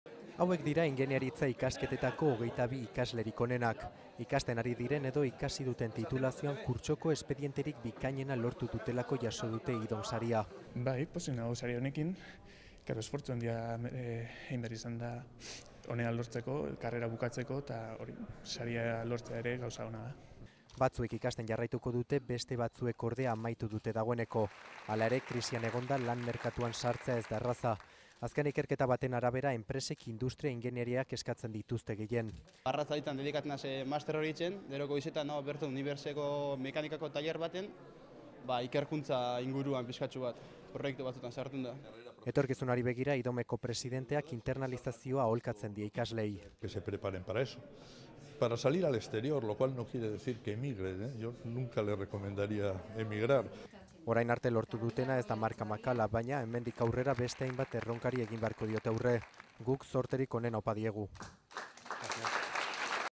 Bilboko Eskola Teknikoko 22 ikaslek 'Idom' saria jaso dute
Ingeniaritza ikasketetako 22 ikaslerik onenak dira. Ikasten ari diren edo ikasi duten titulazioan, kurtsoko espedienterik bikainena lortu dute.